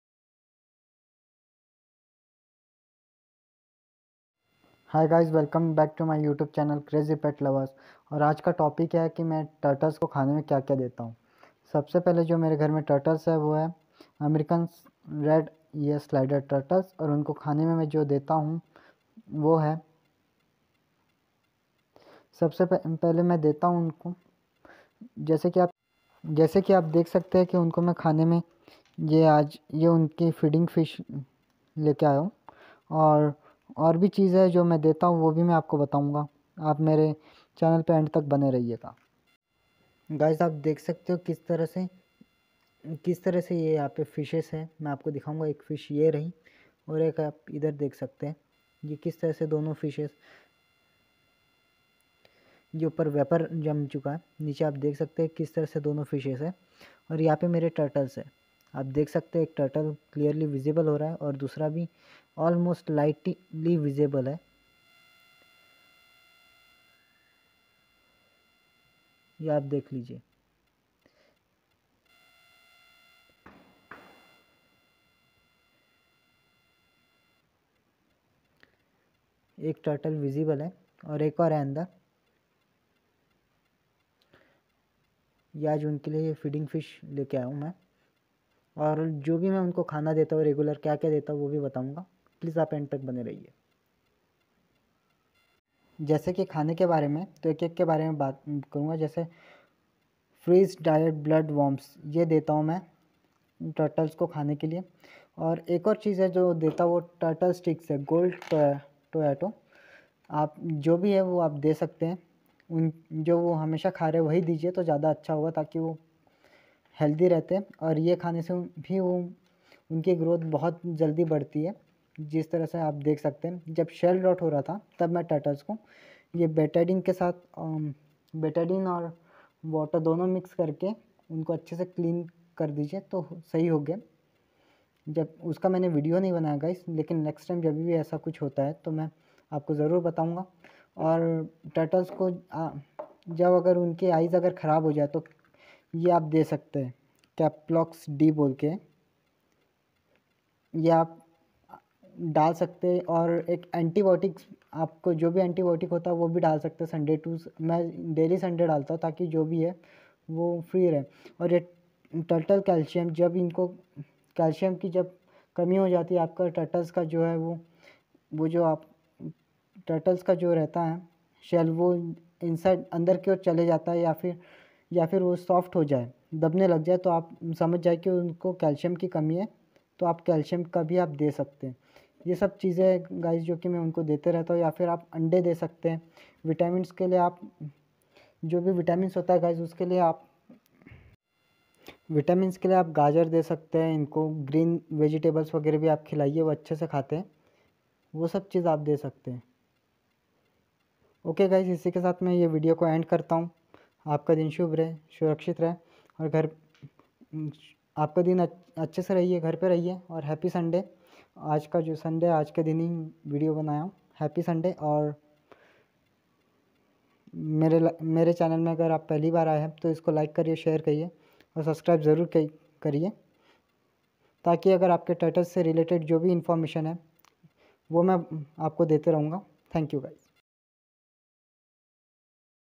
Today's special turtle 🐢🐢 feeding food🐟🐟
In brief discussion about Turtles feeding food and the diseases of the turtle.